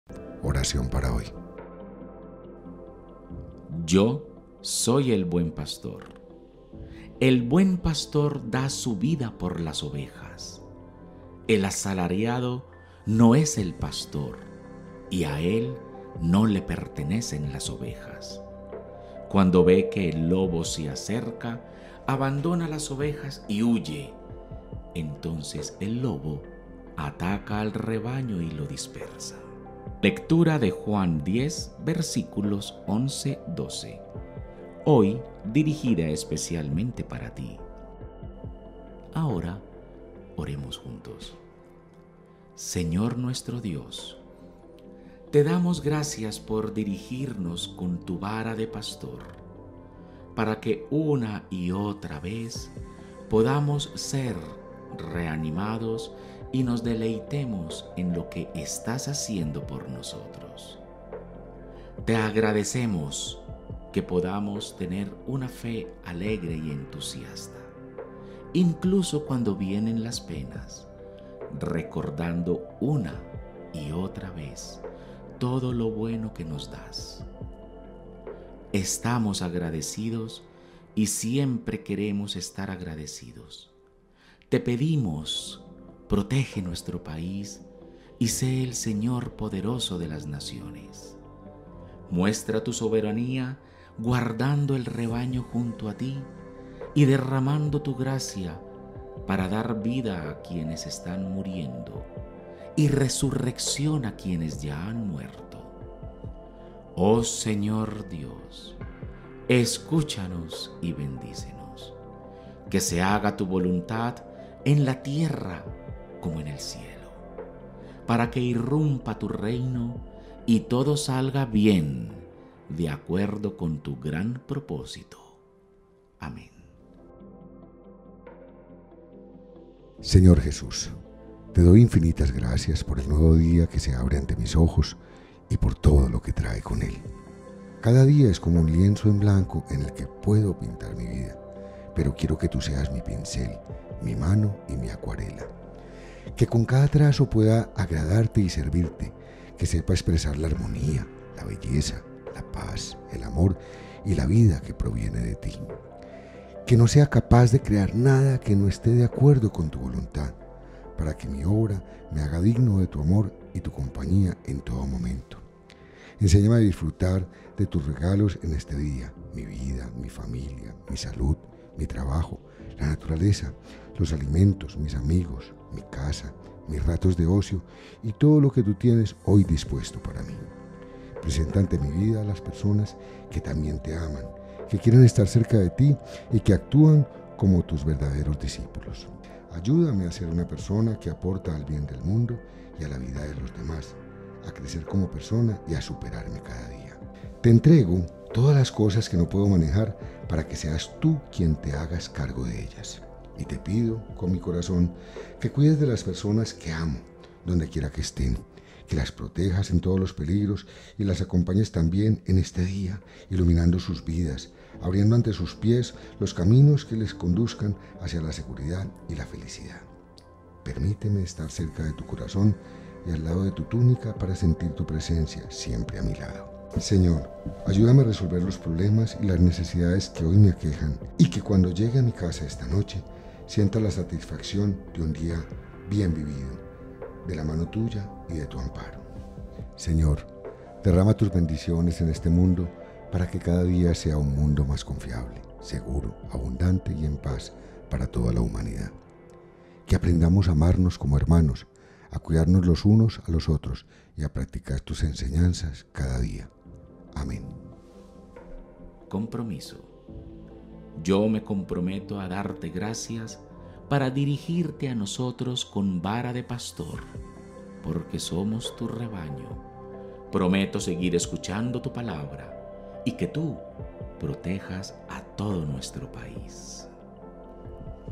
Oración De La Mañana De Hoy, Lunes 10 De Enero De 2022 - Cosmovision
Cosmovision te invita a compartir con nosotros la Oración de la mañana de hoy Lunes 10 De Enero del 2022.

Señor Jesús, te doy infinitas gracias por el nuevo día que abre ante mis ojos y por todo lo que trae con Él.
Cada día es como un lienzo en blanco en el que puedo pintar mi vida, pero quiero que tu seas mi pincel, mi mano y mi acuarela.
Que con cada trazo pueda agradarte y servirte, que sepa expresar la armonía, la belleza, la paz, el amor y la vida que provienen de ti.
Que no sea capaz de crear nada que no esté de acuerdo con tu voluntad.
Para que mi obra me haga digno(a) de tu amor y tu compañía en todo momento.
Enséñame a disfrutar de tus regalos en este día: mi vida, mi familia, mi salud, mi trabajo, la naturaleza, los alimentos, mis amigos, mi casa, mis ratos de ocio, y todo lo que tú tienes hoy dispuesto para mí.
Presenta ante mi vida a las personas que también te aman, que quieren estar cerca de ti hoy y que actúan como tus verdaderos discípulos.
Ayúdame a ser una persona que aporta al bien del mundo y de la vida de los demás, a crecer como persona y a superarme cada día.
Te entrego todas las cosas que no puedo manejar para que seas tu quien te hagas cargo de ellas.
Y te pido con mi corazón que cuides de las personas que amo donde quiera que estén, que las protejas de todos los peligros y las acompañes también en este día iluminando sus vidas, abriendo ante sus pies los caminos que los conducen hacia la seguridad y la felicidad.
Permíteme estar cerca de tu corazón y al lado de tu túnica, para sentir tu presencia siempre a mi lado.
Señor, ayúdame a resolver los problemas y las necesidades que hoy me aquejan y a llegar a casa en la noche con la satisfacción de un día vivido de tu mano y bajo tu amparo.
Derrama tus bendiciones en este mundo, para que cada día sea un mundo más confiable, seguro, abundante y en paz para toda la humanidad.
Que aprendamos a amarnos como hermanos, a cuidar unos de otros y a practicar tus enseñanzas cada día.
Amén.